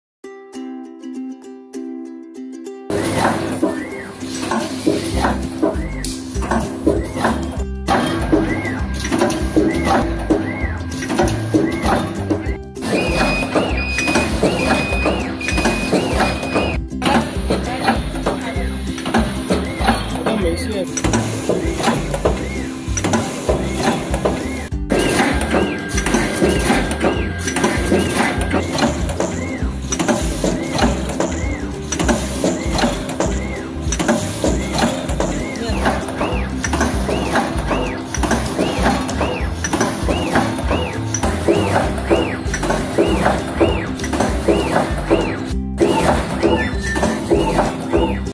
AUTOMATIC SIU MAI MAKING MACHINE 自动广式烧麦制作设备
AUTOMATIC SIU MAI MACKING MACHINE

The siu mai making machine is used for Cantonese siu mai making. This machine has dough pressing, dough cutting, stuffing filling, model pressing, siu mai output functions.

The automatic siu mai making machine one hour can make 1200 to 3600 pieces. The machine body is use 304 stainless steel, make sure the food safety.